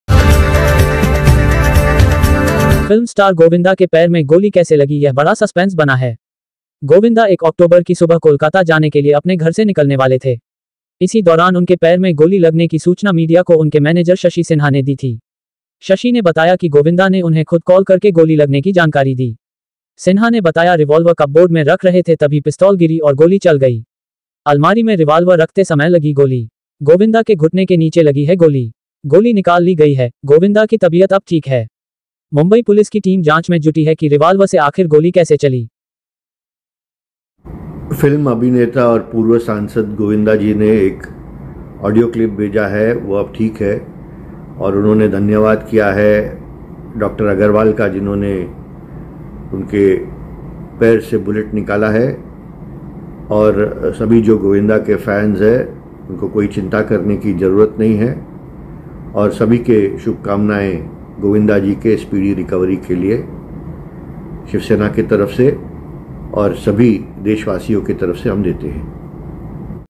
फ़िल्म स्टार गोविंदा के पैर में गोली कैसे लगी बड़ा सस्पेंस ? मुंबई पुलिस जांच में जुटी
फ़िल्म स्टार गोविंदा के पैर में गोली कैसे लगी यह बड़ा सस्पेंस बना है।
 गोविंदा 1 ऑक्टोबर की सुबह कोलकाता जाने के लिए अपने घर से निकलने वाले थे,
इसी दौरान उनके पैर में गोली लगने की सूचना मीडिया को उनके मैनेजर शशि सिन्हा ने दी थी।
शशि ने बताया कि गोविंदा ने उन्हें खुद कॉल कर के गोली लगने की जानकारी दी।

सिन्हा ने बताया रिवॉल्वर कपबोर्ड में रख रहे थे तभी पिस्तौल गिरी और गोली चल गई।अलमारी में रिवाल्वर रखते समय लगी गोली।

गोविंदा के घुटने के नीचे लगी है गोली।गोली निकाल ली गई है,गोविंदा की तबीयत अब ठीक है।
मुंबई  पुलिस की टीम जांच में जुटी है कि रिवाल्वर से आखिर गोली कैसे चली।